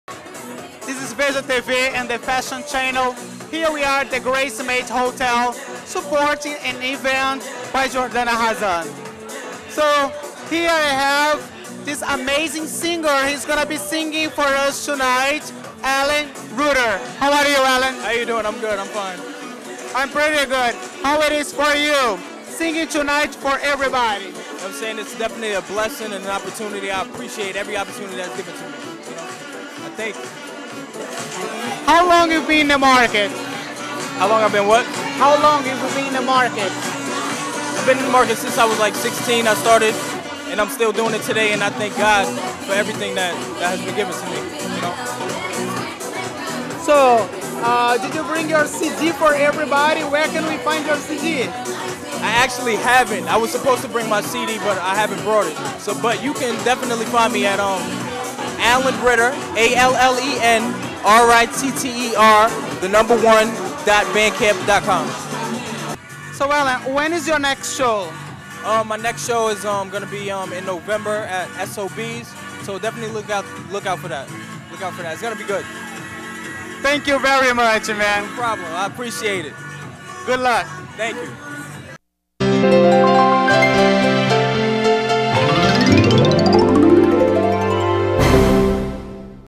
Allen Ricter Untapped Fashion Show
George Robert interview the singer Allen Ricter at Untapped Fashion Show in New York.